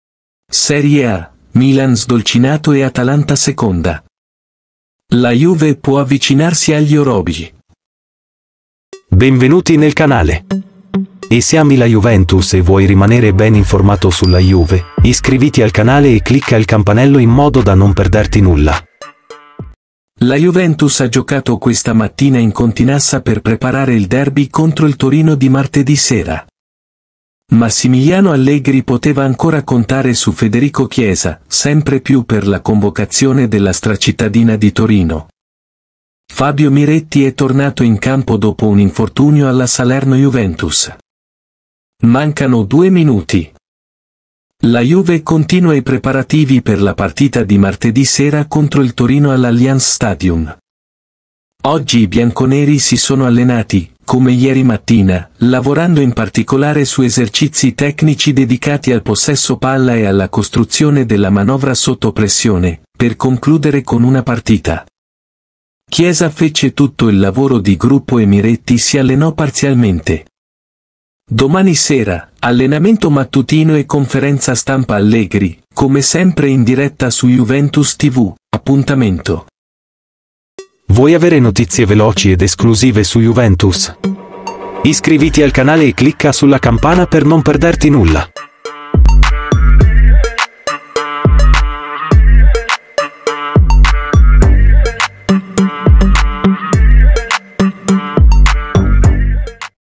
Serie A, il Milan piega l'Atalanta ed è secondo. La Juve può avvicina..(NOTIZIE DA JUVENTUS)
La Juventus ha giocato questa mattina in Continassa per preparare il derby contro il Torino di martedì sera. Massimiliano Allegri poteva ancora contare su Federico Chiesa, sempre più per la convocazione della Stracittadina di Torino. Fabio Miretti è tornato in campo dopo un infortunio alla Salerno-Juventus.
Mancano due minuti. La Juve continua i preparativi per la partita di martedì sera contro il Torino all'Allianz Stadium.

Oggi i bianconeri si sono allenati, come ieri mattina, lavorando in particolare su esercizi tecnici dedicati al possesso palla e alla costruzione della manovra sotto pressione, per concludere con una partita.

Chiesa fece tutto il lavoro di gruppo e Miretti si allenò parzialmente.

Domani sera: allenamento mattutino e conferenza stampa Allegri, come sempre in diretta su Juventus TV. Appuntamento